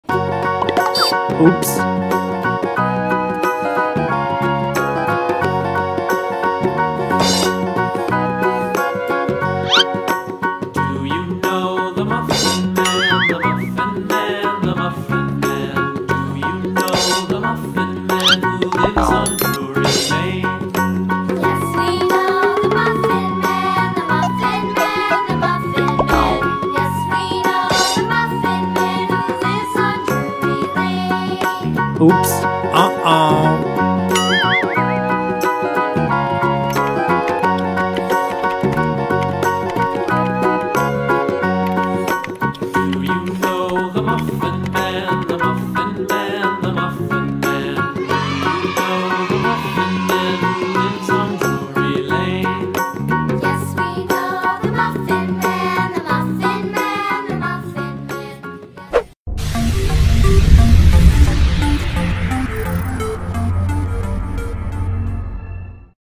Mobil Truk pasir Anak anak_LUCU Truknya Warna-Warni, pasirnya warna Pink
Mainan mobil mobilan truk pasir yang menyenangkan, Mobil truk pasir anak anak, Truk pasirnya Warna warni dan ada tiga ya teman teman, seru banget Loh.!